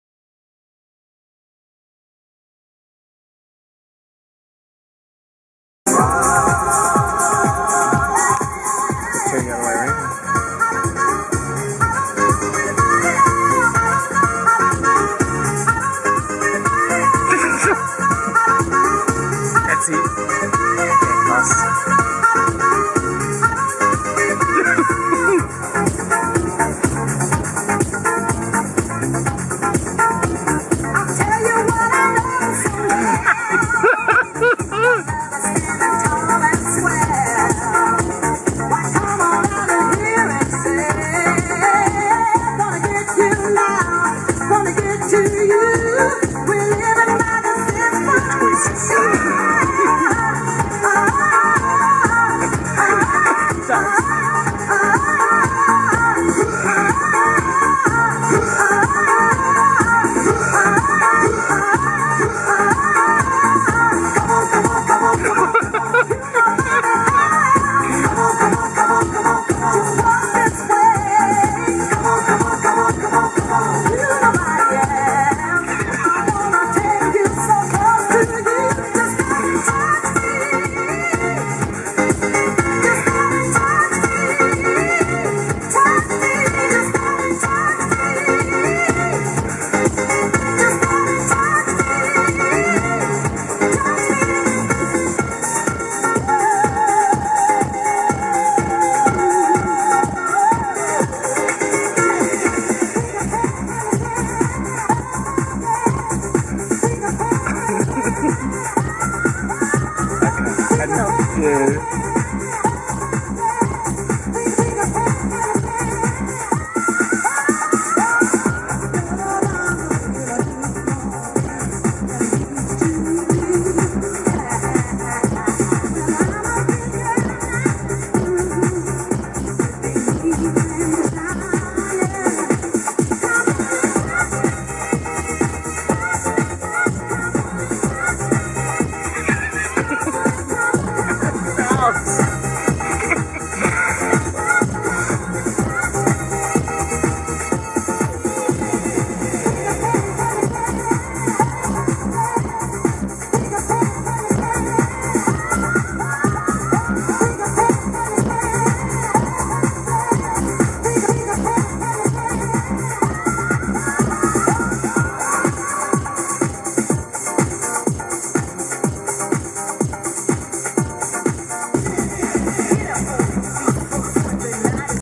Dancing Man
We noticed the air man was moving in sync with the music we were playing in the car and decided to film it for a laugh on our recent trip to New Zealand